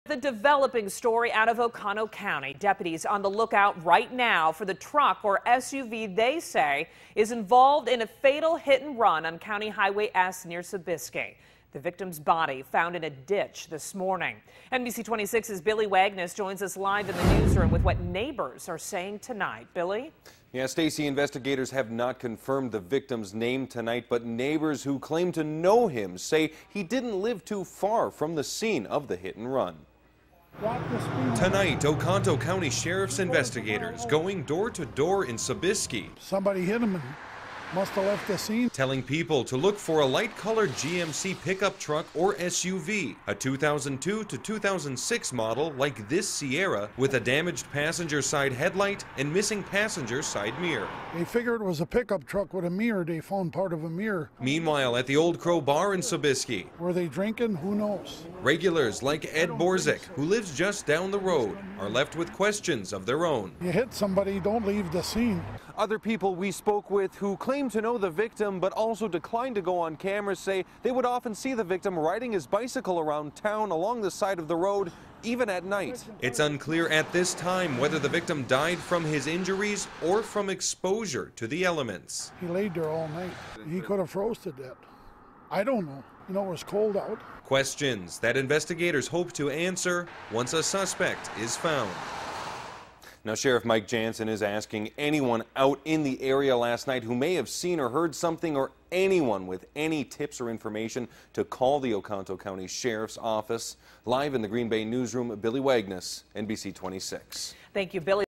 Man found dead in Sobieski ditch, believed to be victim of hit-and-run
SOBIESKI, WI -- A man was found dead Tuesday morning in a ditch on the north side of County Highway S, west of Sobieski, Oconto County Sheriff Michael Jansen said.